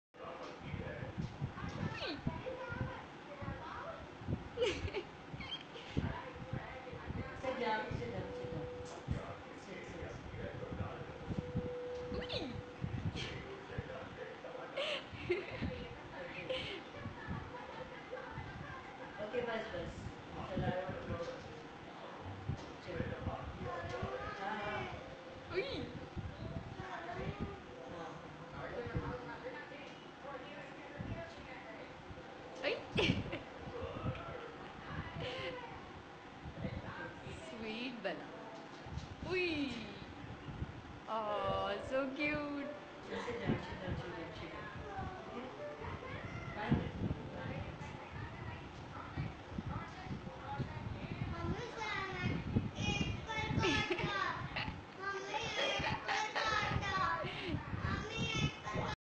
Dont Work Just Love Me / Pet Behaving Like a Kid
She Doesn't want me to work